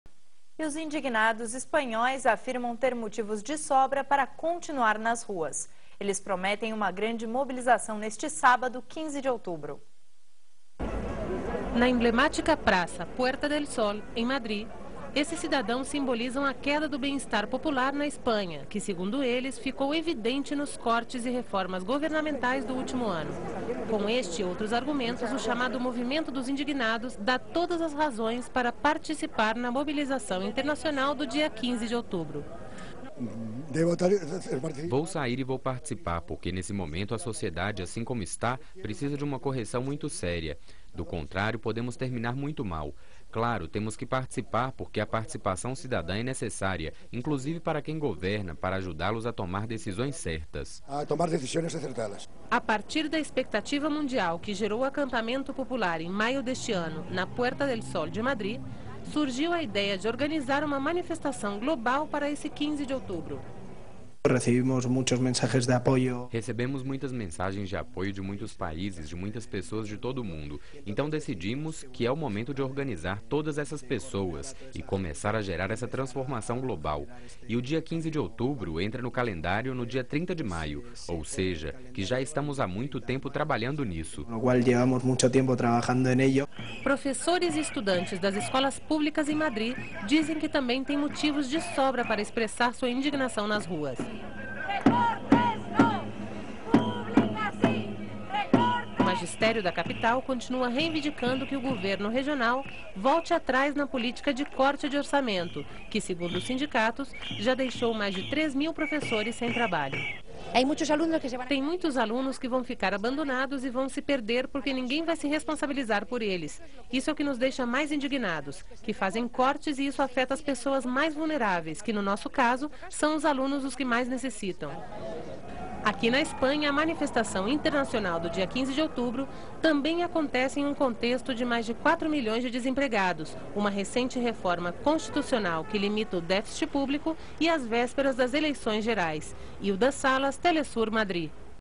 Indignados espanhóis continuam protestando
Os Indignados espanhóis prometem uma grande mobilização nesse sábado 15 de outubro. Professores e estudantes de Madri também expressam sua indignação nas ruas na capital. Cerca de três mil professores vão ficar sem trabalho pela nova reforma educativa. teleSUR